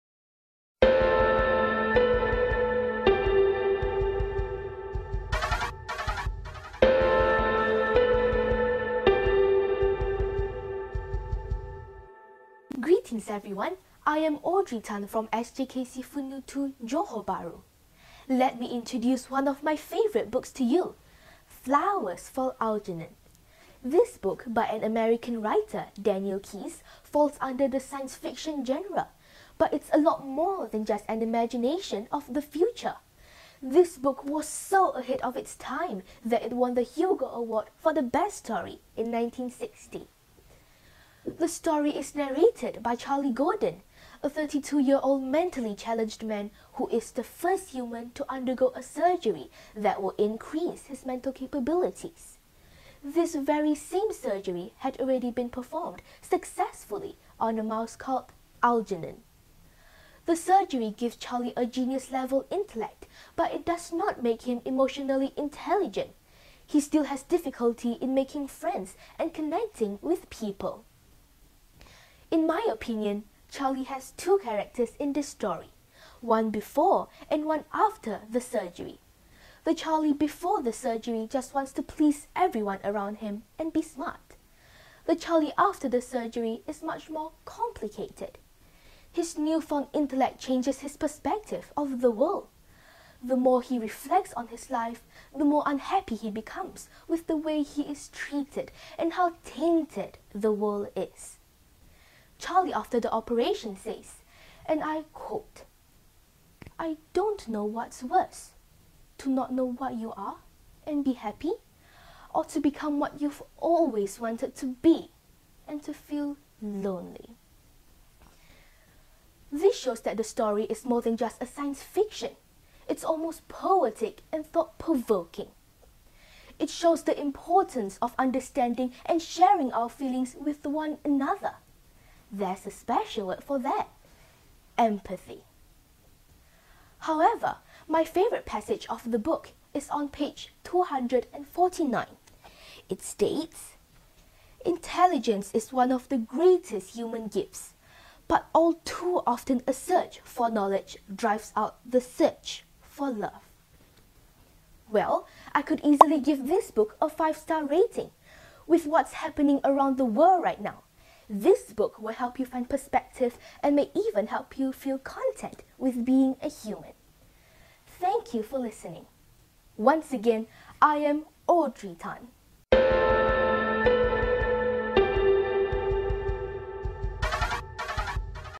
Reading Duo Book Review - Flowers for Algernon by Audrey Tan, 12 years old SJKC Foon Yew 2
E-Stream Waves 2021, Universiti Teknologi Petronas

Reading Duo Book Review

Flowers for Algernon

By Audrey Tan

12 years old

SJKC Foon Yew 2, Johor Bahru.